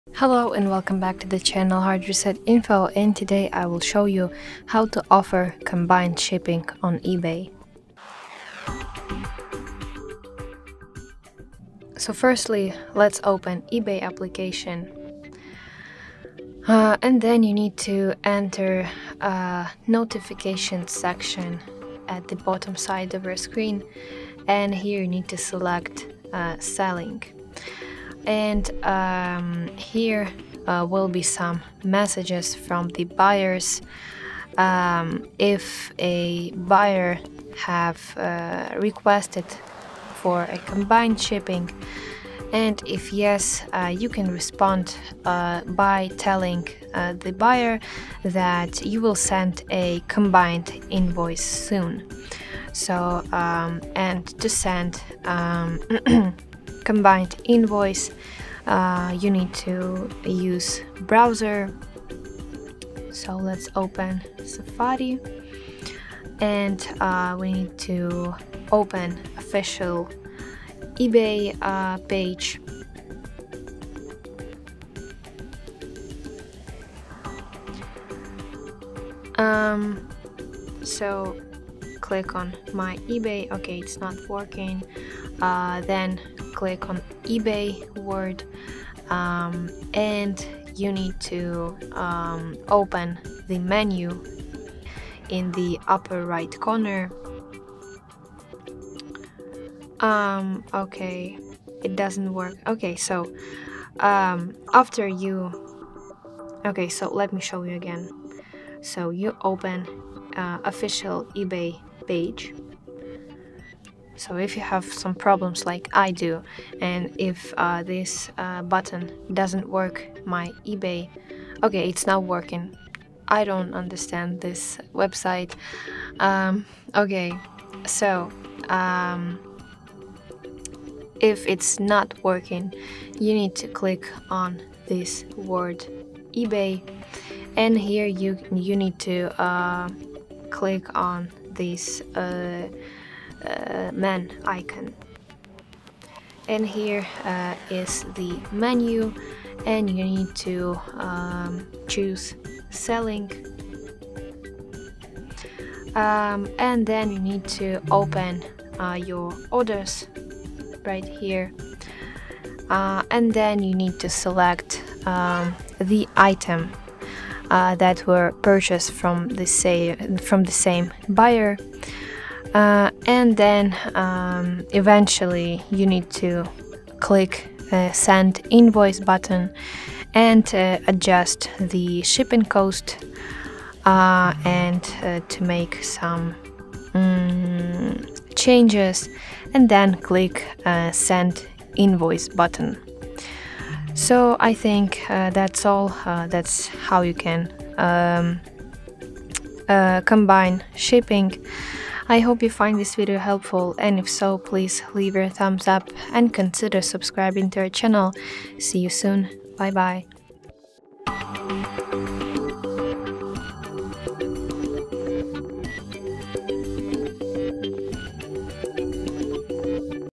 How to Offer Combine Shipping on eBay
Want to offer combined shipping to your buyers on eBay? In this video, we show you exactly how to respond to a combined shipping request and send a combined invoice using the eBay website. While you can view buyer messages in the Selling section of the eBay app, you’ll need to use a browser to access your orders and send the invoice. We walk you through how to select items from the same buyer, adjust the shipping cost, and finalize the invoice.

Perfect for sellers looking to save buyers money and simplify the checkout process.

How to send a combined invoice on eBay?
Can I offer combined shipping through the eBay app?
Where to adjust shipping cost for multiple items on eBay?

⏱️ TIMESTAMPS:
0:00 Intro
0:14 Open eBay app and check Selling messages
0:40 Open browser and go to eBay website
1:19 Access menu and go to Selling
2:36 Open Orders and select items from the same buyer
2:56 Click Send Invoice and adjust shipping
3:13 Finalize and send combined invoice